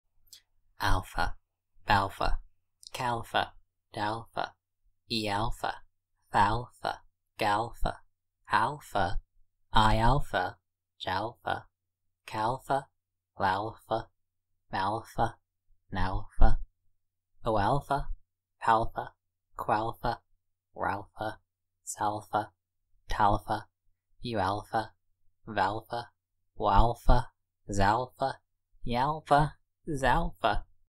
100% accurate NATO phonetic alphabet pronunciation guide
Use these pronunciations when spelling out letters over the phone and you'll be amazed at how easy you are to understand!